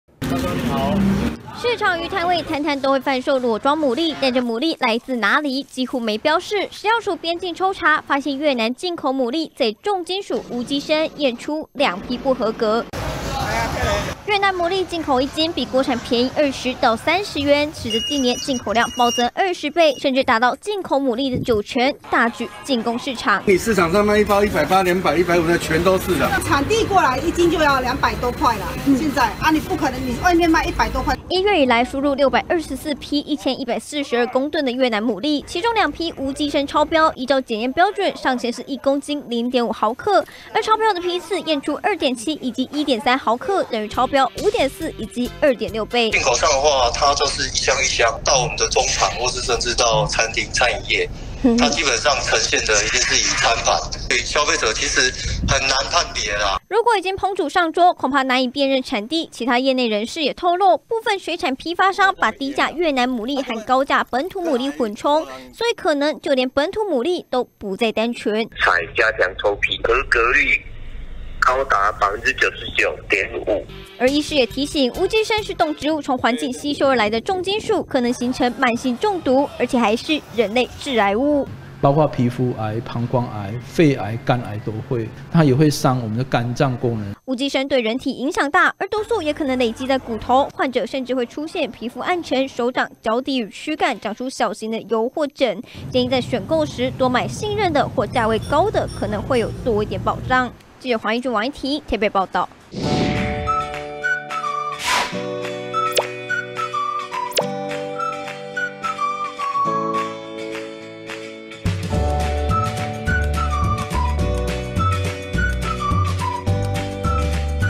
【每日必看】驗出致癌"無機砷" 越南牡蠣混充本土流入市面? 20230515
食藥署已經在邊境!查獲2批越南進口牡蠣，重金屬無機砷含量超標，引發婆婆媽媽恐慌，尤其越南進口牡蠣一斤 比國產便宜 20到30元，近年來已經大舉攻佔國內市場，甚至有業內士透露，部分標榜本土的廠商會混著賣，民眾根本分不出台灣牡蠣還是越南牡蠣!而長期食用含砷牡蠣，恐怕會致癌!不過食藥署也強調目前沒有讓超標牡蠣，流入市面!